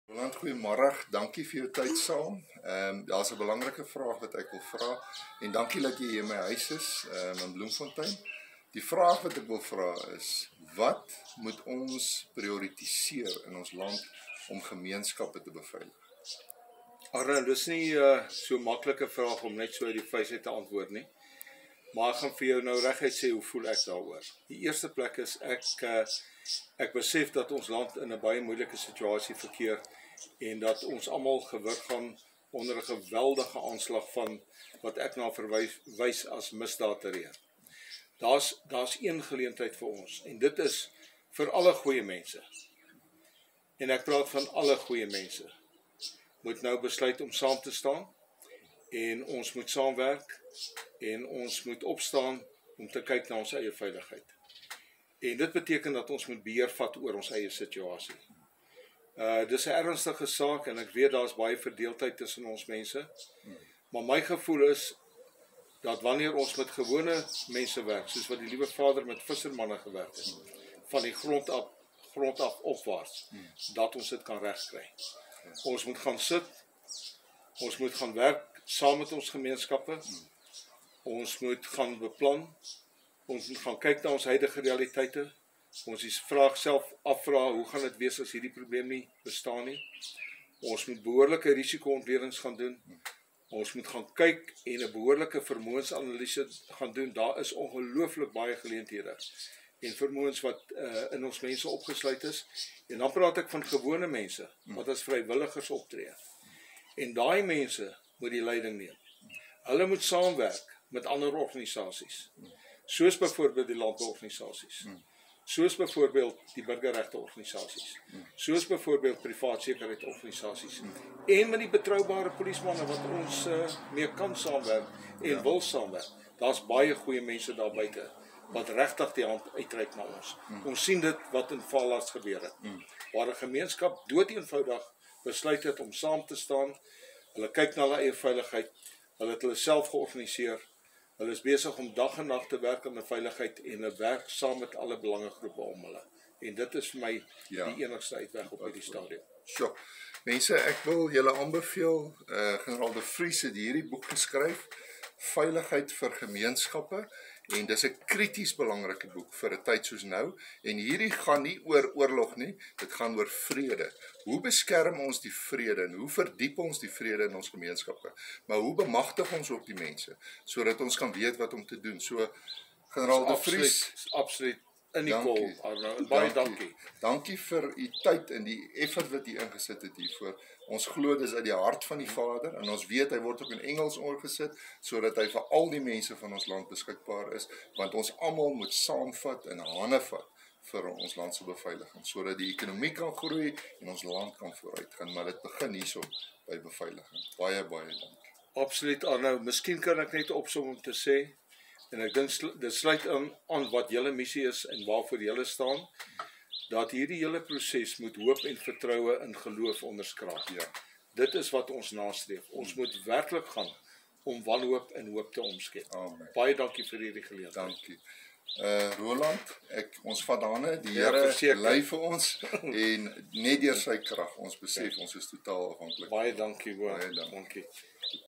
Genl. Roland de Vries se belangrike woorde | Dr. Arno van Niekerk
Die beveiling van ons gemeenskappe is van kardinale belang, maar dit vra samewerking deur ALLE rolspelers. Ons vat hande saam met Genl de Vries om te werk aan 'n geïntegreerde beveilingingsmodel wat ALLE gemeenskappe bevoordeel. Kry sy boek!